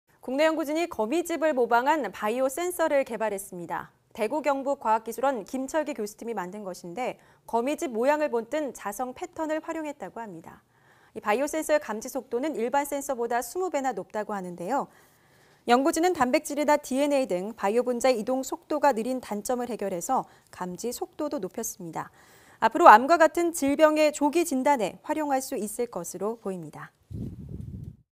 거미집 모방한 바이오센서 개발…20배 빨리 감지 / YTN 사이언스
국내 연구진이 거미집을 모방한 바이오 센서를 개발했습니다.

대구경북과학기술원 김철기 교수팀이 만든 것인데 거미집 모양을 본뜬 자성패턴을 활용했다고 합니다.

이 바이오 센서의 감지속도는 일반 센서보다 20배나 높다고 하는데요.

연구진은 단백질이나 DNA 등 바이오 분자의 이동 속도가 느린 단점을 해결해 감지속도를 높였는데요.

앞으로 암과 같은 질병의 조기 진단에 활용할 수 있을 것으로 보입니다.